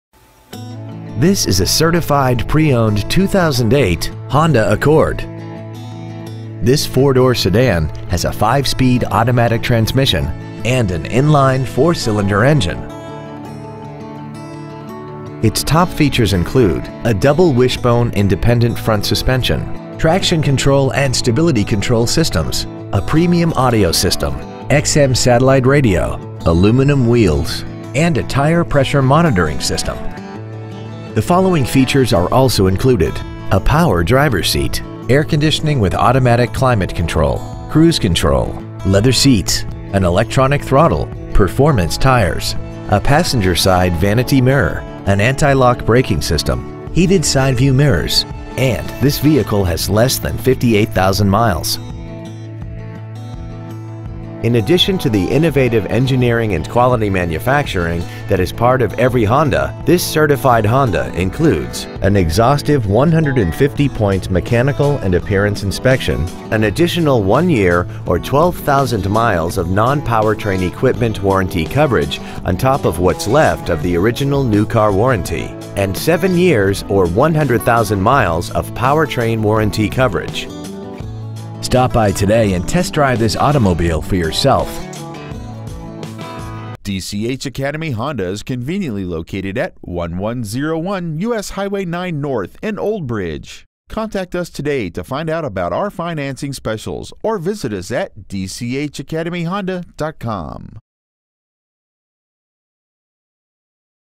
Pre-Owned 2008 Honda Accord Cpe Old Bridge NJ 08857
Year : 2008
 Make : Honda
 Model : Accord Cpe
 Engine : Gas I4 2.4L/144
 Trans . : Automatic
 Exterior : Taffeta White
 Miles : 57,312
 Interior : Ivory
 Stock : AH30744A

 1101 US Highway 9 North